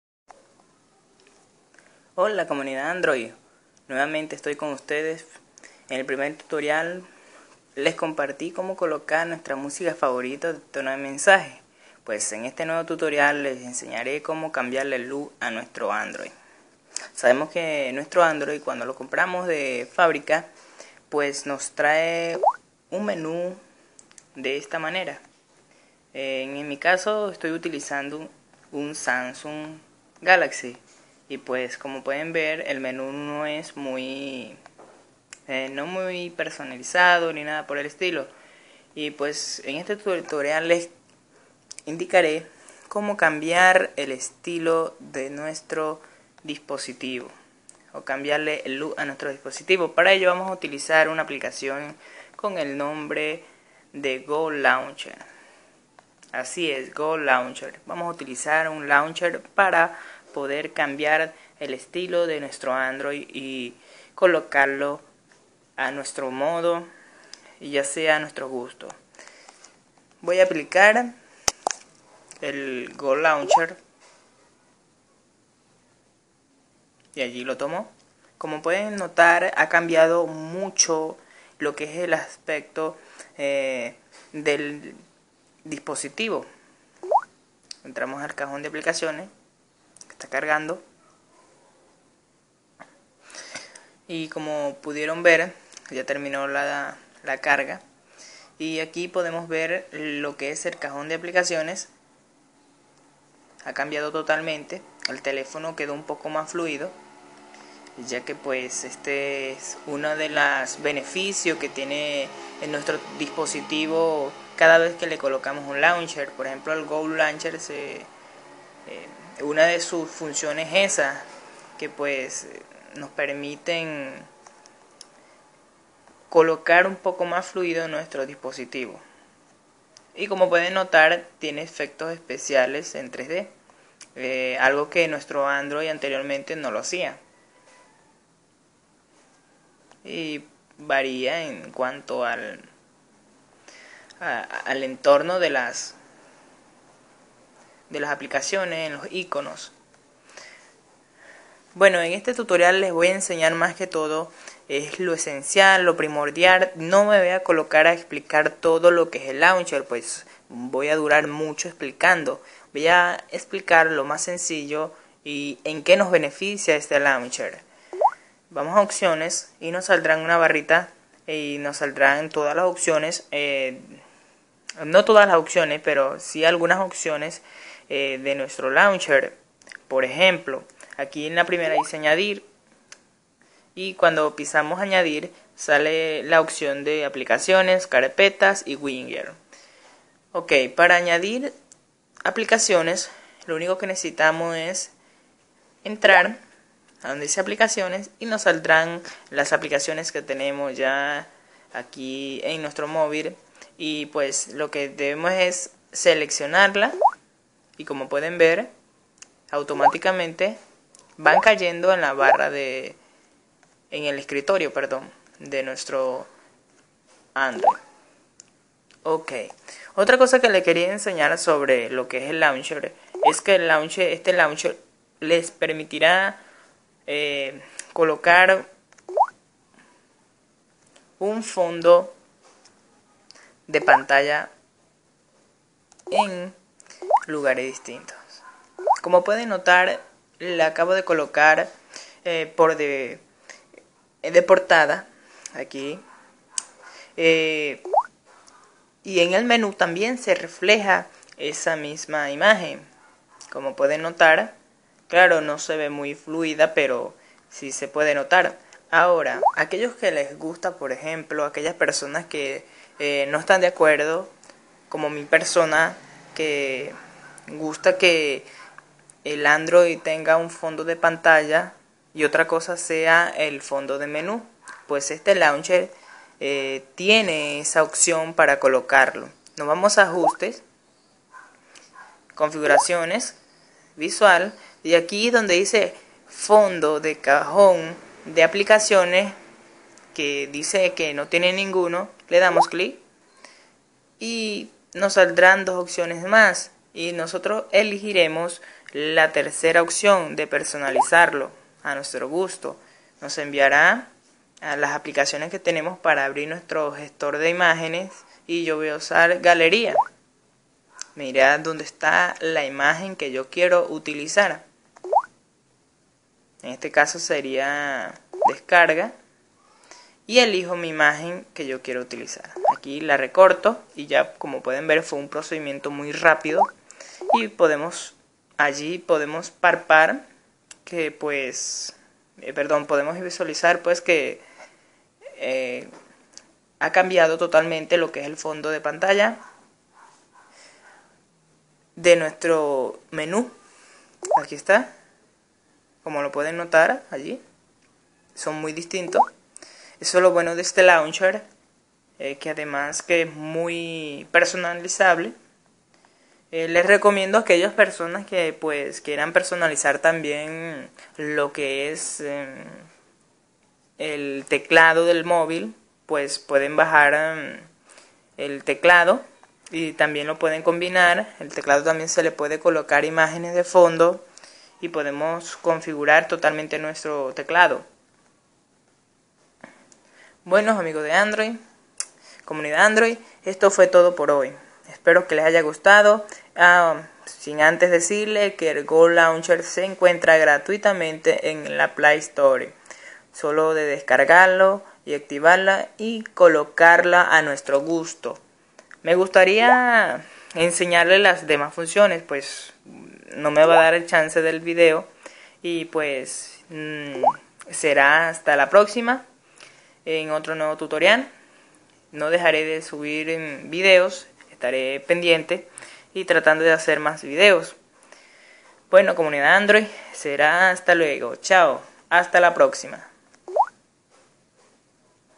¿Como Cambiar el Estilo a Nuestro Android?, Utilizando un Launcher.
Hola comunidad android :)
en este tutorial les enseñare como cambiar el lok a nuestro dispositivo android
utilizando el Go launcher, que se puede descargar gratis por el play store.

MySword para Android con Modulos de Biblias en español